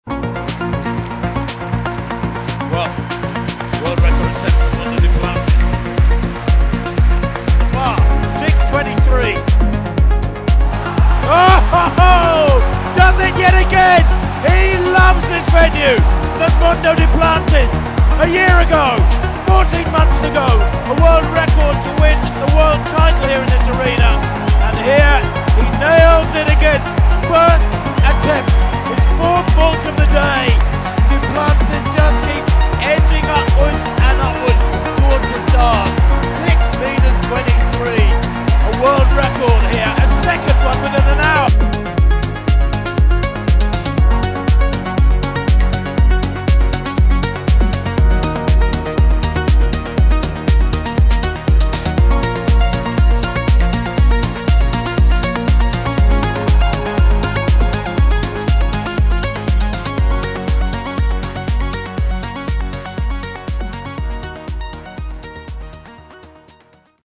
Armand DUPLANTIS - 6.23m - World Record (MiX)
Armand DUPLANTIS - 6.23m WR (MiX)
Wanda Diamond League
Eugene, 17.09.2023